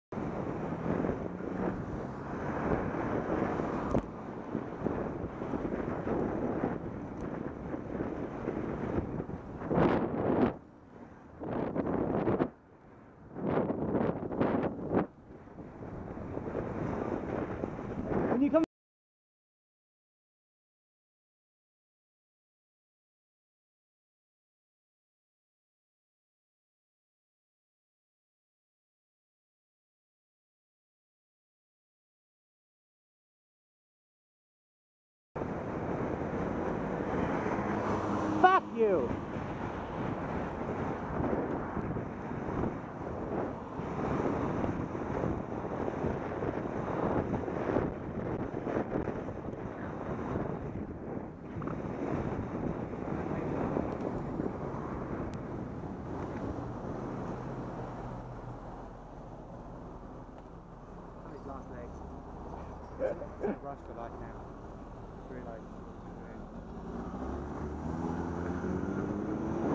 Old man buzzes me and friend on way to work. (CA 4ZTE253 Silver PT Cruiser)
Beginning of the video the driver of the PT Cruiser is approaching quickly. We start our merge because of the park cars and he does not act too pleased. Then later on down the road he buzzes us only to come to a stop right ahead. If I would have realized his left brake light was out I would have given him some grief. The car right behind him passes properly.